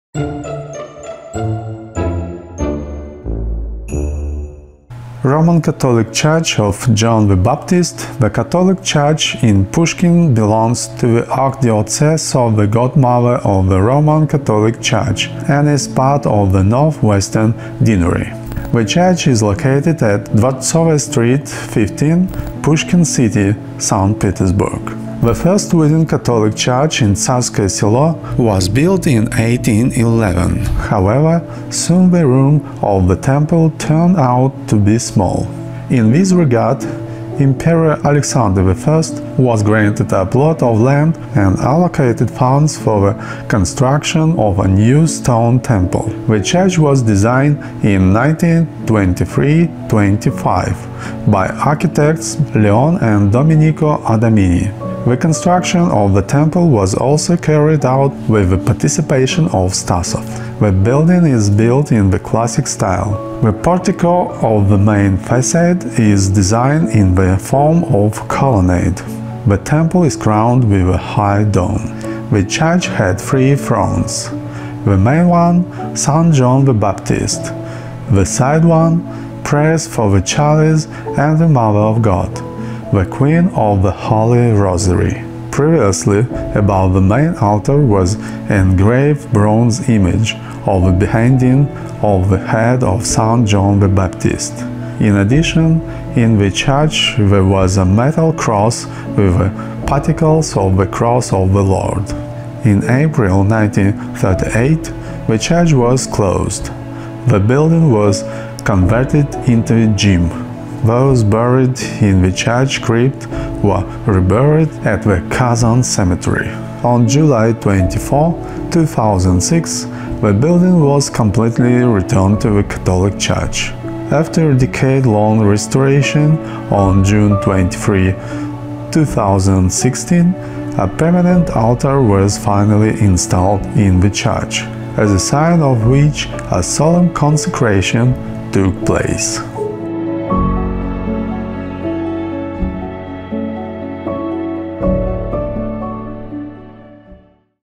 Roman Catholic Church of John the Baptist
Roman Catholic Church of John the Baptist---the Catholic church in Pushkin belongs to the Archdiocese of the God Mother of the Roman Catholic Church and is part of the North-Western Deanery.
The church is located at Dvortsovaya Street, 15  Pushkin city (Saint Petersburg)
The first wooden Catholic church in Tsarskoye Selo  was built in 1811. However, soon the room of the temple turned out to be small.
In this regard, Emperor Alexander I was granted a plot of land and allocated funds for the construction of a new stone temple. The church was designed in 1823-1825 by the architects Leone and Domenico Adamini. The construction of the temple was also carried out with the participation of V. P. Stasov.
The building is built in the classical style. The portico of the main facade is designed in the form of a colonnade. The temple is crowned with a high dome.
The church had three thrones: the main one — St. John the Baptist; the side ones-Prayers for the Chalice and the Mother of God, the Queen of the Holy Rosary.
Previously, above the main altar was an engraved bronze image of the Beheading of the head of St. John the Baptist. In addition, in the church there was a metal cross with particles of the Cross of the Lord.
In April 1938, the church was closed. The building was converted into a gym. Those buried in the church crypt were reburied at the Kazan cemetery.
On July 24, 2006, the building was completely returned to the Catholic Church. After a decade-long restoration, on June 23, 2016, a permanent altar was finally installed in the church, as a sign of which a solemn consecration took place.